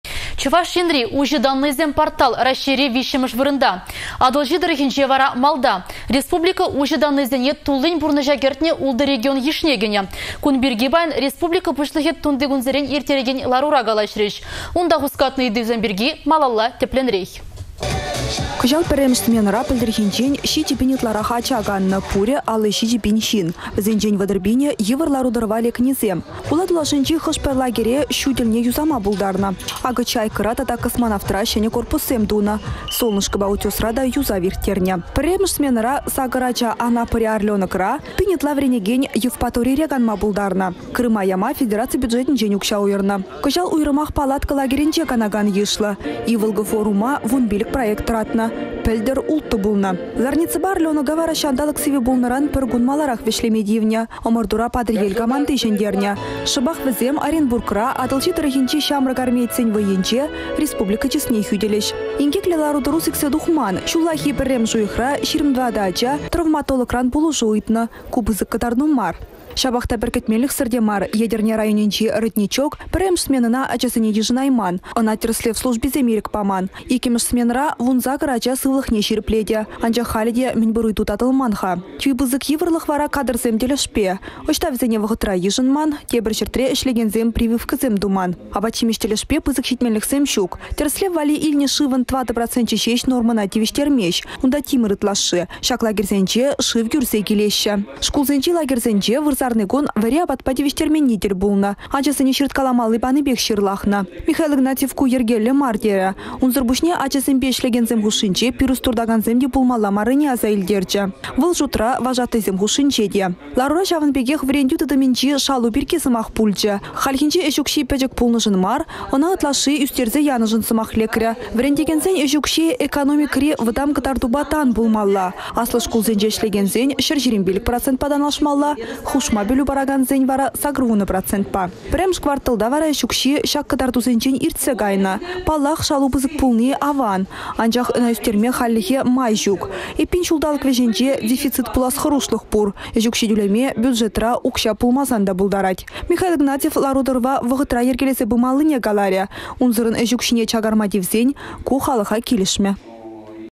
Лагерьсенче 57 пин ача каннă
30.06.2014
Çĕртме уйăхĕнче республикăра пурăнакан 57 пин ача лагерьте е ытти кану вырăнĕсенчен сывлăхне çирĕплетнĕ. Кун пирки паян республика пуçлăхĕ тунти кунсерен ирттерекен ларура калаçрĕç.